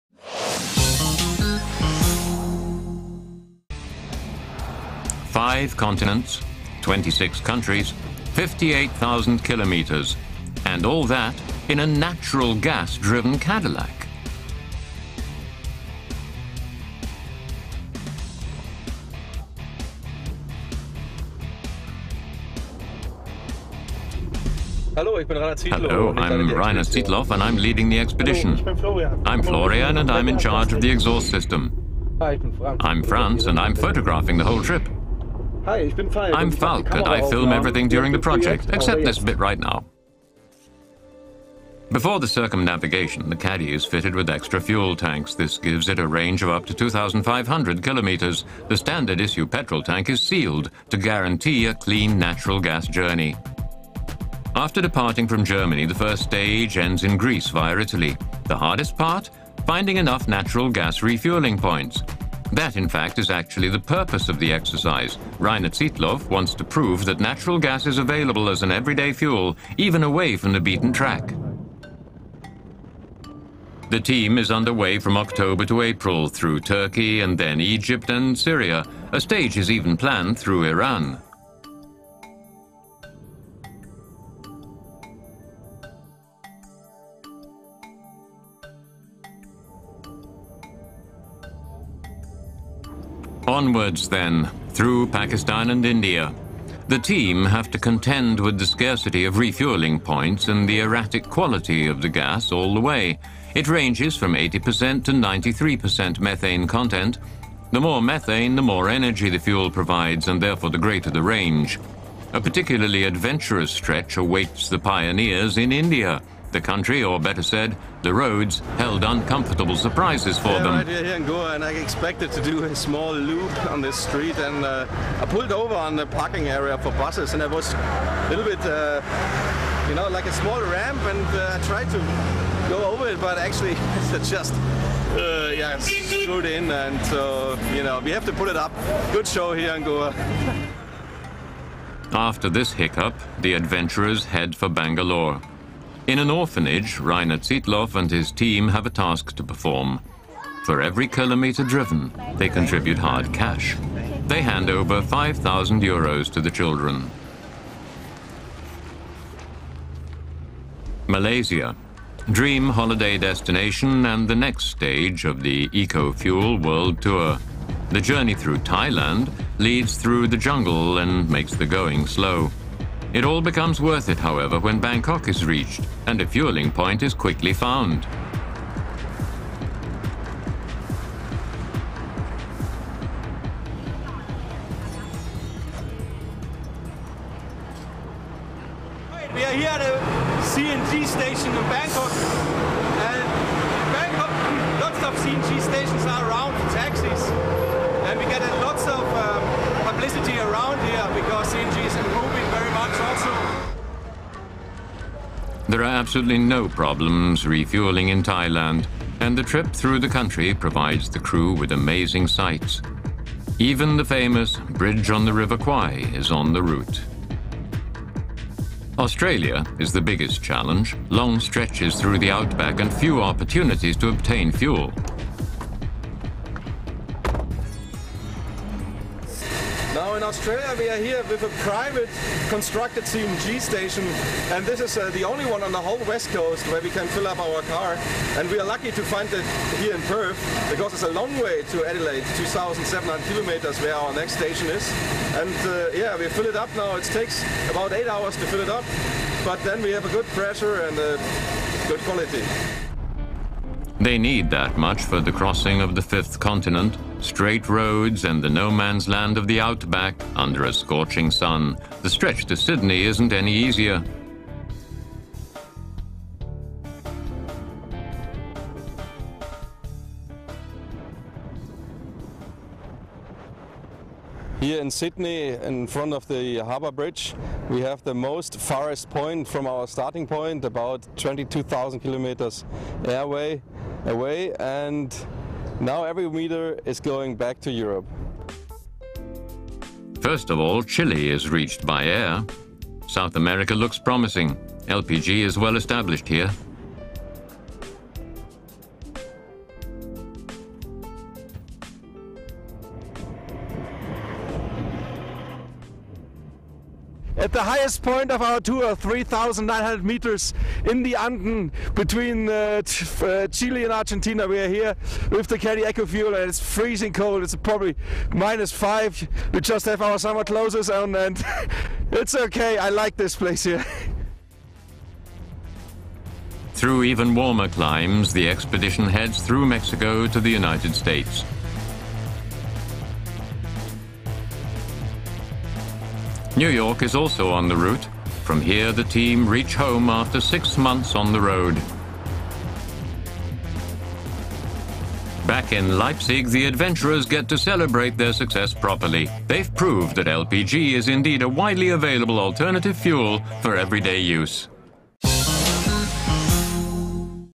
VW Caddy gas expedition
85.000 kilometres in a gas driven VW caddy - a very special expedition through 26 countries.